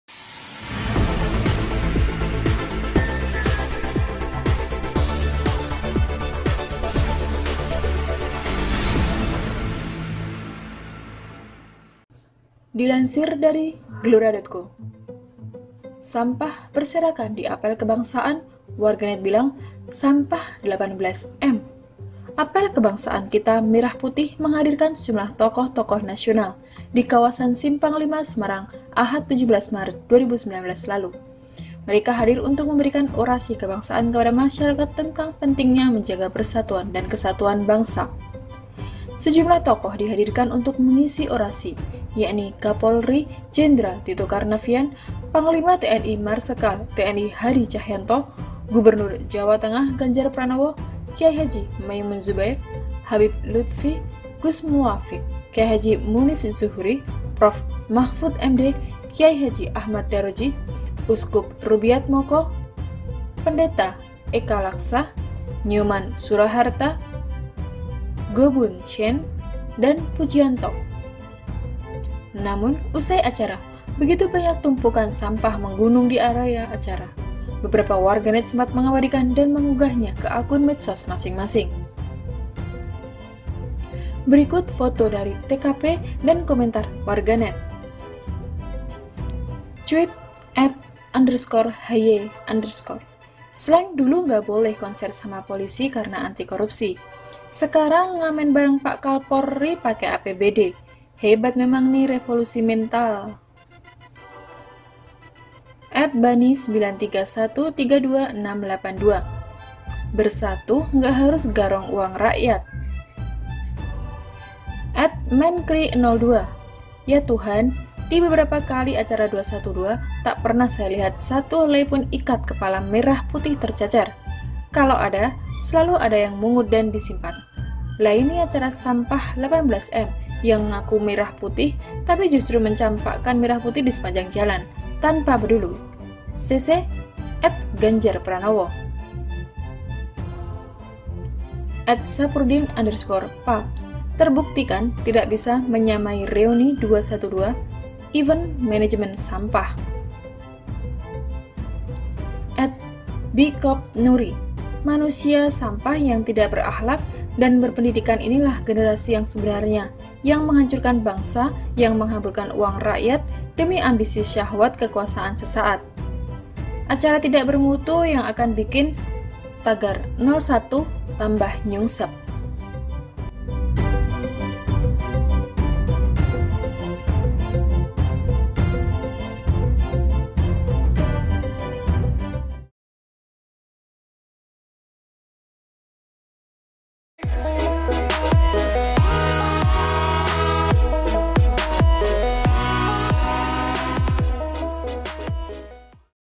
BI4NG S4MP4H ! APEL K3B4NGS4AN 18M CM BIKIN SAMPAH ! TERNYATA INI YG BIKIN NYUNGSEP
Sampah Berserakan di Apel Kebangsaan, Warganet: Sampah 18 M

GELORA.CO - Apel Kebangsaan Kita Merah Putih menghadirkan sejumlah tokoh-tokoh nasional, di kawasan Simpanglima Semarang, Ahad 17 Maret 2019.

Mereka hadir untuk memberikan orasi kebangsaan kepada masyarakat tentang pentingnya menjaga persatuan dan kesatuan bangsa.

Sejumlah tokoh dihadirkan untuk mengisi orasi yakni Kapolri Jenderal Tito Karnavian, Panglima TNI Marsekal TNI Hadi Tjahjanto, Gubernur Jawa Tengah Ganjar Pranowo, KH. Maimun Zubair, Habib Luthfi, Gus Muwafiq, KH Munif Zuhri, Prof Mahfud MD, KH Ahmad Daroji, Uskup Rubiatmoko (Keuskupan Agung Semarang), Pendeta Eka Laksa (PGI), Nyoman Suraharta (PHDI), Go Boen Tjien (Matakin) dan Pujianto (Walubi).

Namun, usai acara, begitu banyak tumpukan sampah menggunung di area acara. Beberapa warganet sempat mengabadikan dan mengunggahnya ke akun medsos masing-masing.